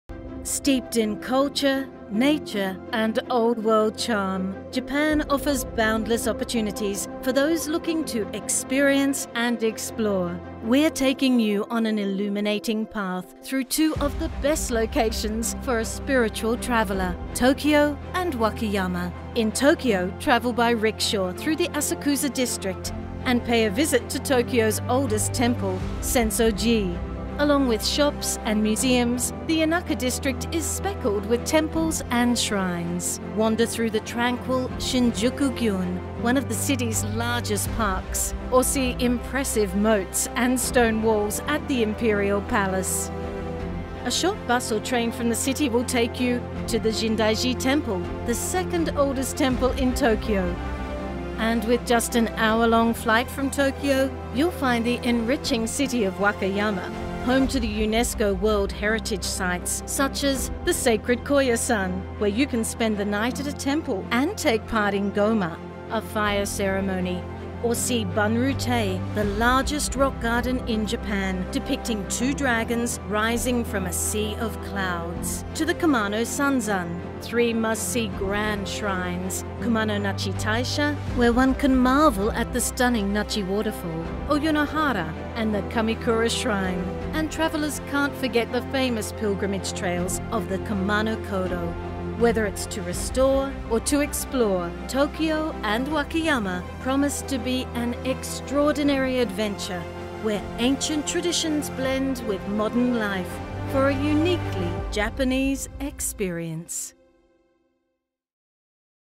A spiritual journey to Japan | Visit Wakayama - The Spiritual Heartland
Ad Paid and presented by Tokyo Metropolitan Government and Wakayama Prefecture. Where Tokyo is considered to be the heart of Japan, Wakayama encapsulates its spirit of the country. With the capital city so well connected by air and rail to the rest of the Japan, Wakayama is just a short trip away for visitors wanting a connection with spirit and nature. The traditional heartland showcases a different side of spiritual Japan, in a way that complements time well spent in Tokyo.